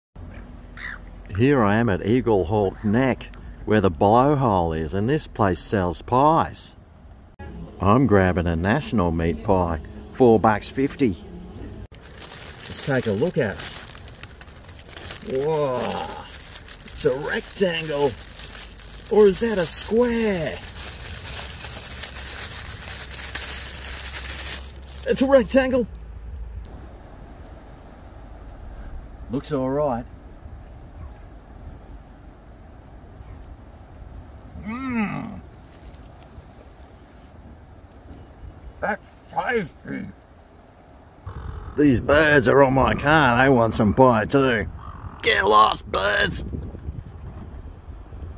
National Meat Pie
at the blow hole in Tasmania and stopped for a national meat pie